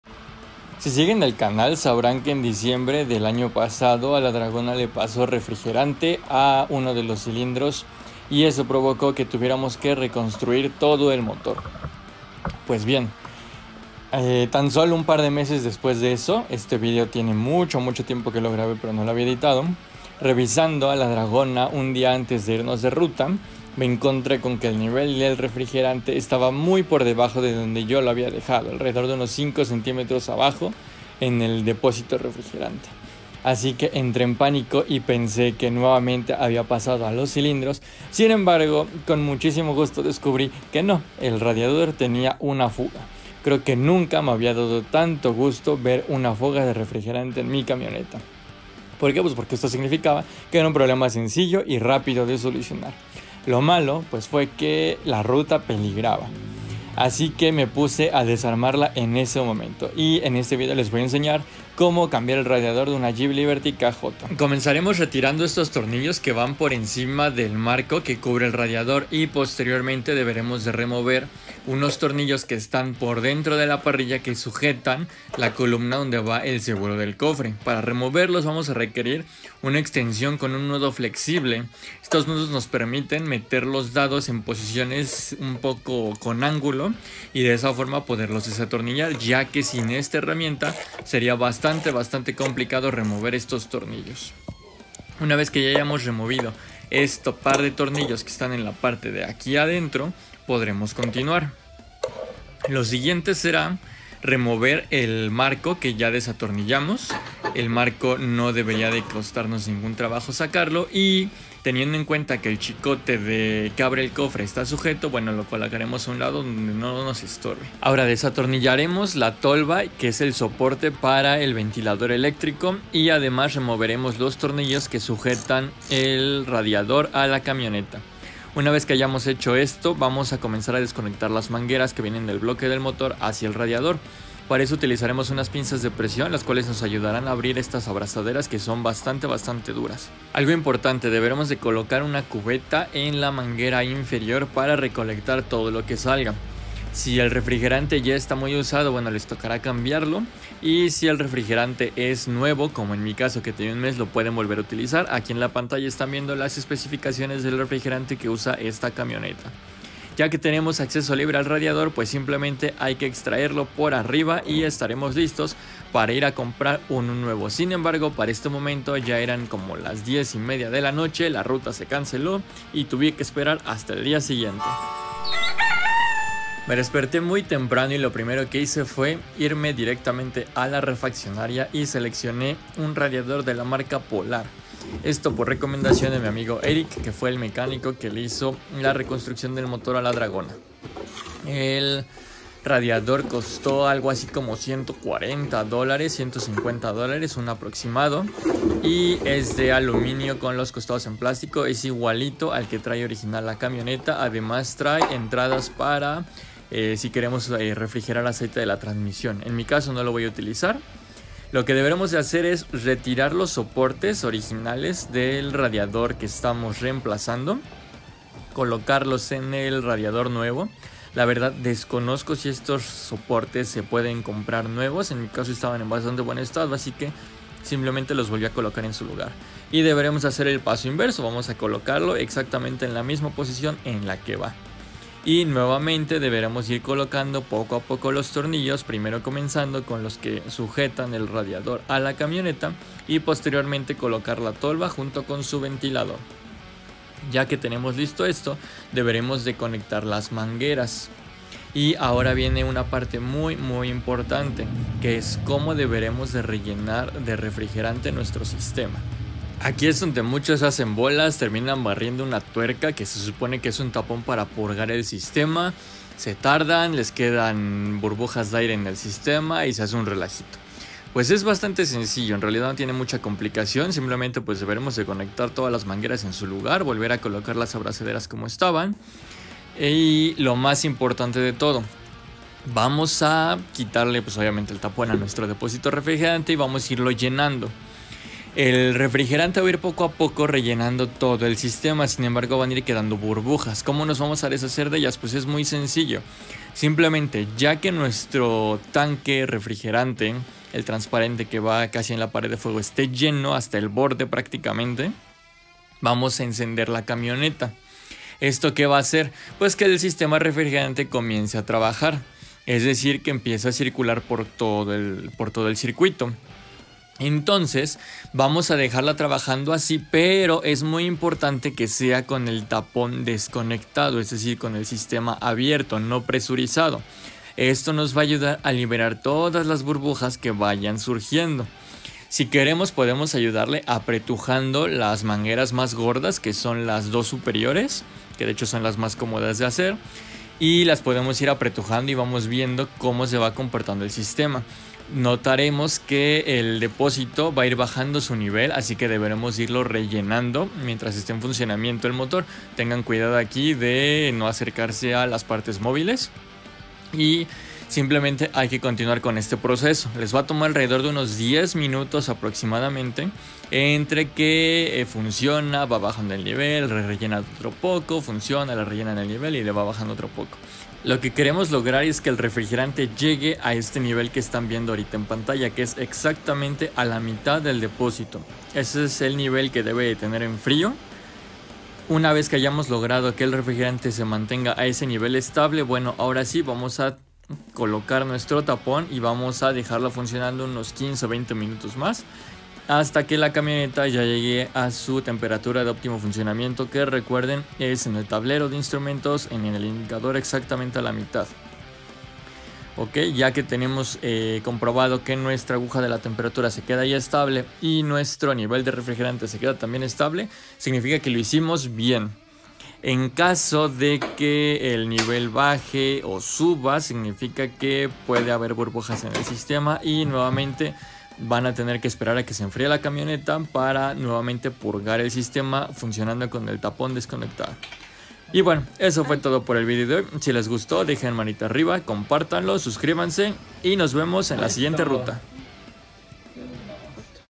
Cómo cambiar el radiador de una Jeep Liberty KJ Cómo purgar el sistema refrigerante
En este video te enseño a cambiar el radiador de tu Liberty además te enseño a purgarlo para sacarle las burbujas de aire que pueden generar un calentamiento de tu motor.

Si tu radiador tiene fugas, lo mejor es cambiarlo.